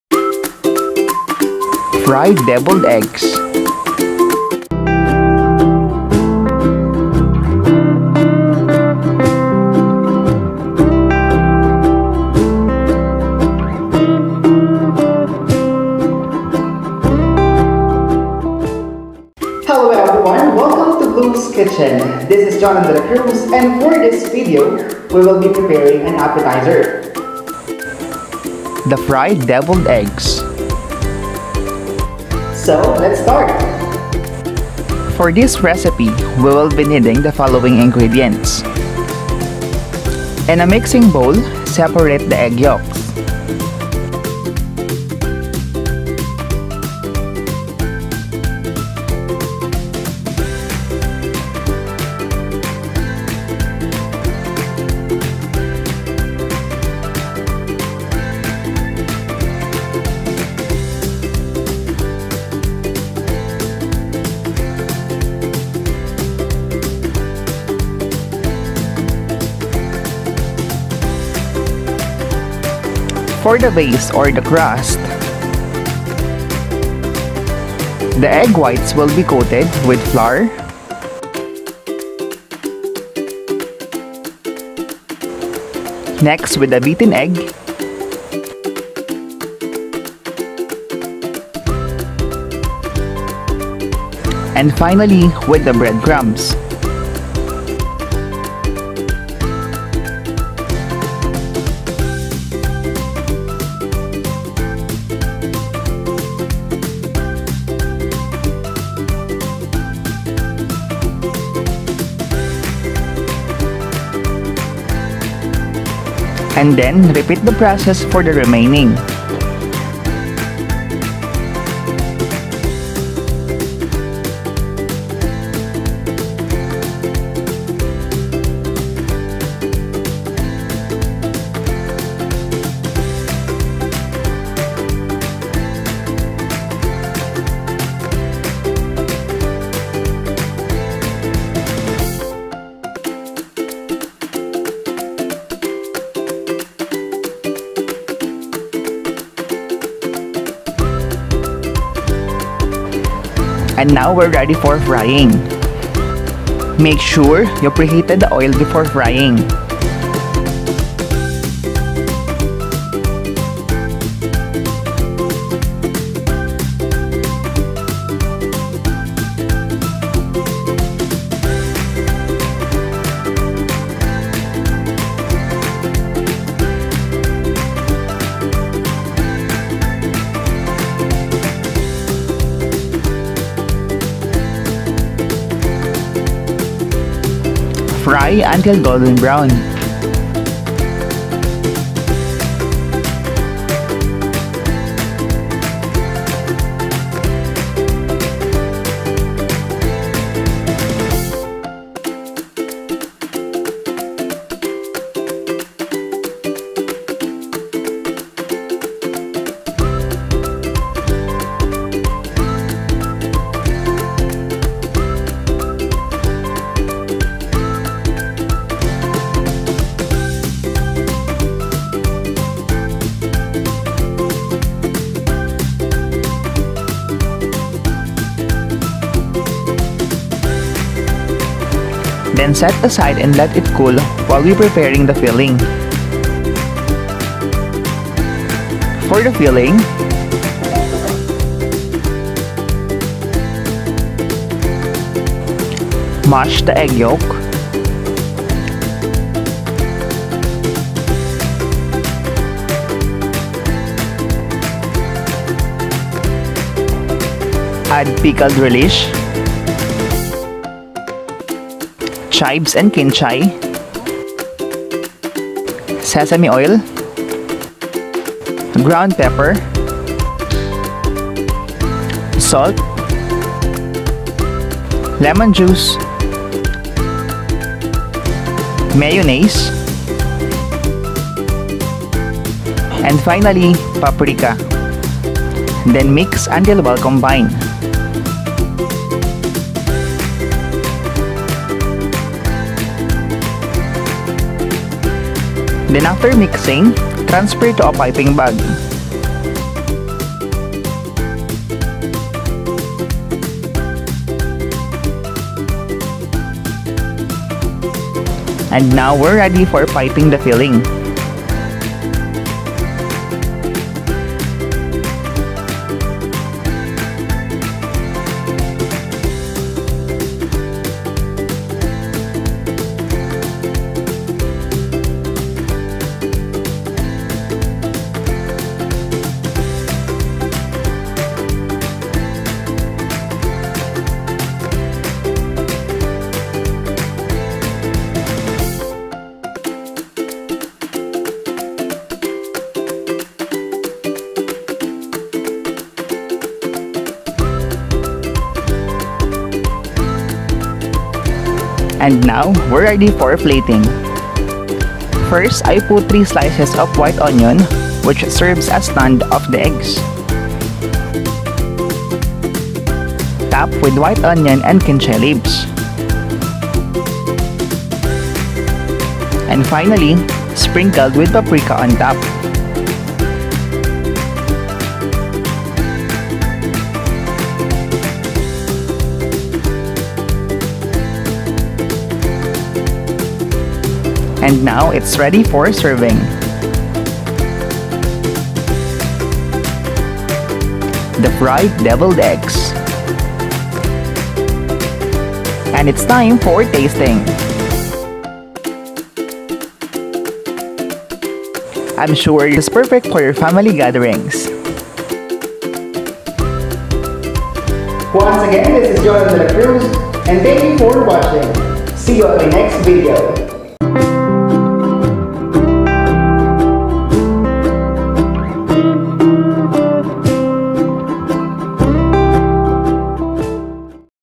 PREPARING APPETIZER | FRIED DEVILLED EGGS
FRIED DEVILLED EGGS
INGREDIENTS:
5 HARD BOILED EGG
FLOUR
BREAD CRUMBS

FOR THE FILLING:
1/4 CUP MAYONNAISE
2 TBSP PICKLE RELISH
SALT
PEPPER
PAPRIKA
LEMON JUICE
KINTSAY
SPRING ONION

The Sound / Song in this Video is the property of the Elite Alliance Music and not of the creator.